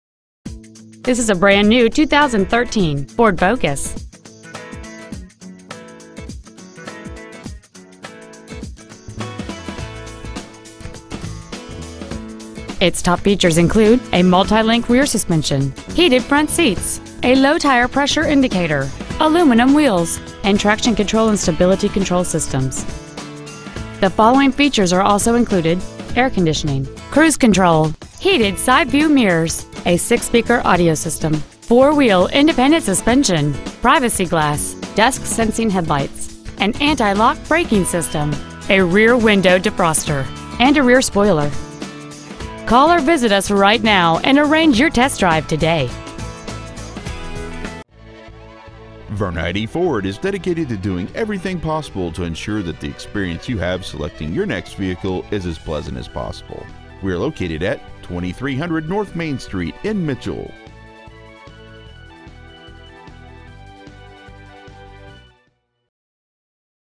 2013 FORD FOCUS Mitchell SD
Year : 2013
 Make : FORD
 Model : FOCUS
 Engine : 2.0L I4 16V GDI DOHC FLEXIBLE FUEL

 Exterior : RUBY RED METALLIC TINTED CLEARCOAT

 Interior : CHARCOAL BLACK / WARM STEEL
 Stock : 92767